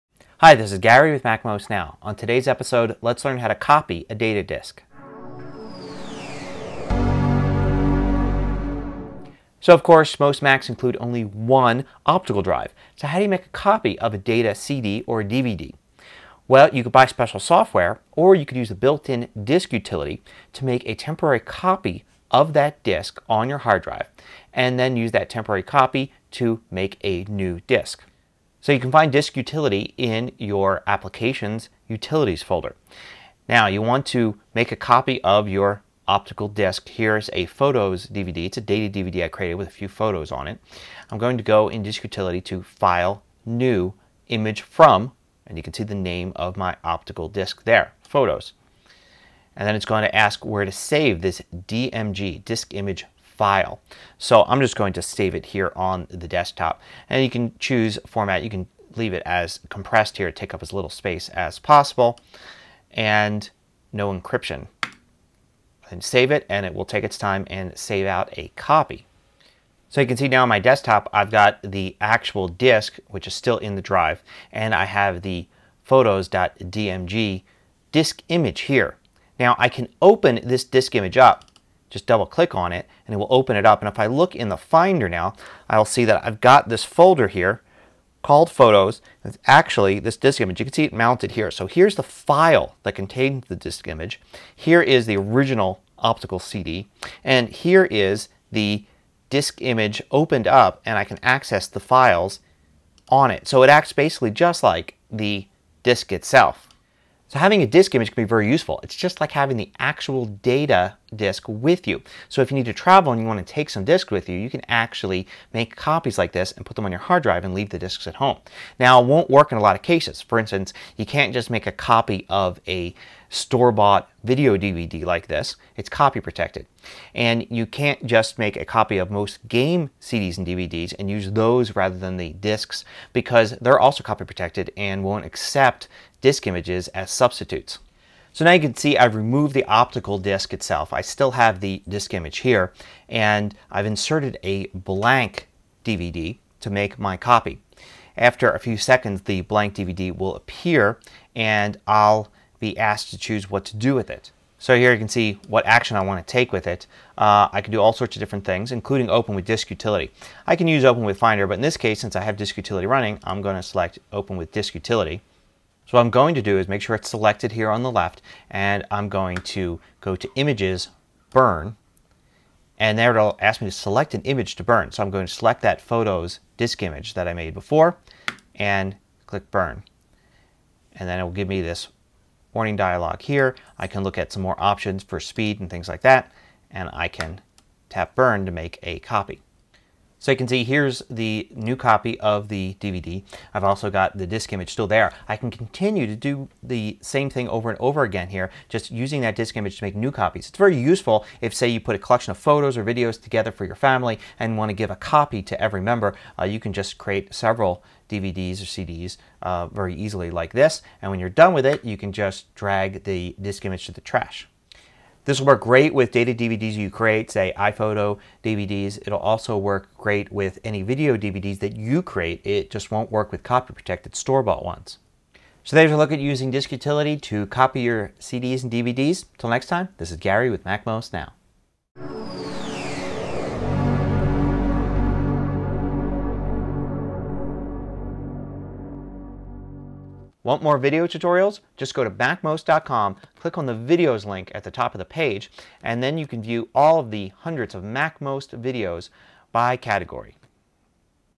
Copying Discs With Disk Utility (MacMost Now 489)
Learn how to make copies of data CDs and DVDs using Disk Utility. With one optical drive in your Mac, you need to create a temporary disk image on your hard drive. Then use that disk image to create physical copies of your disc.